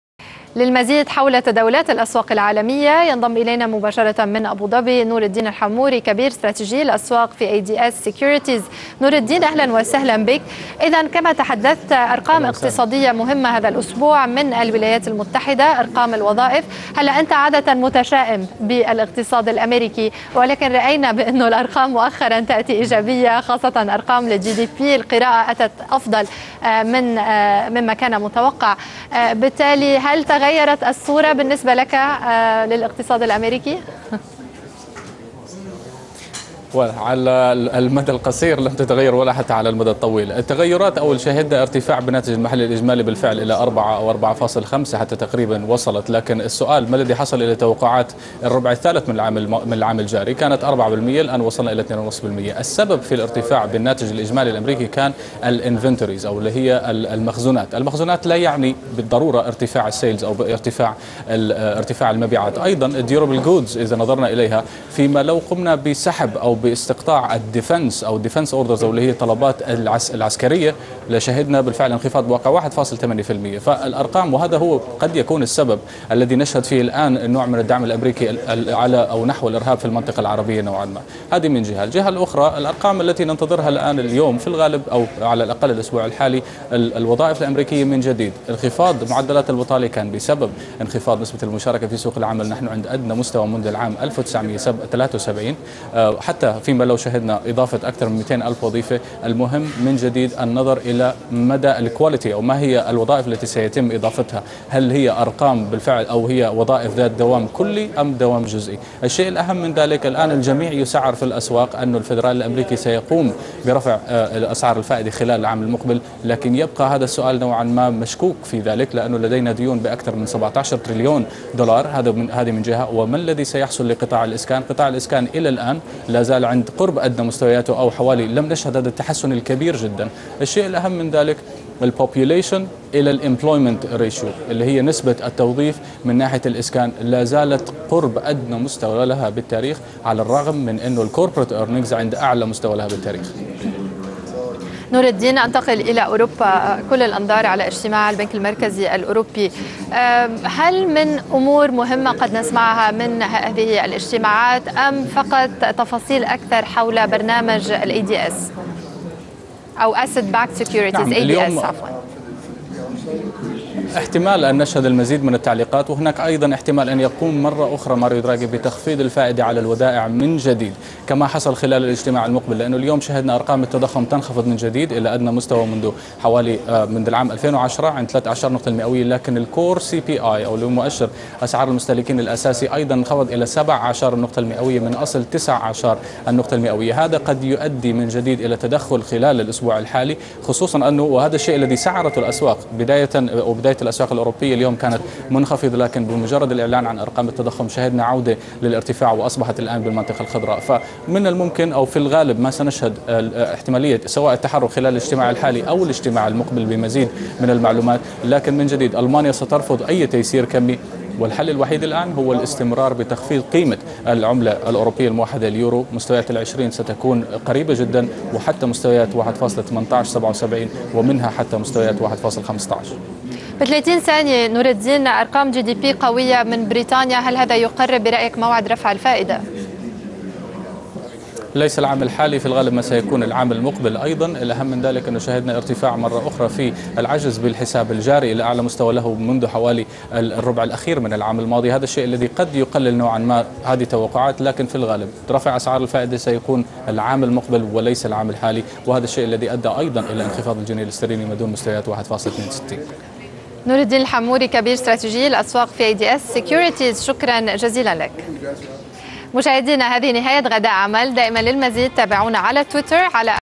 Alarabiya Interview on US Jobs & ECB Decision 30/09/2014
Discussing the US economic figures, US Jobs Report, GBPUSD, ECB decision and EURUSD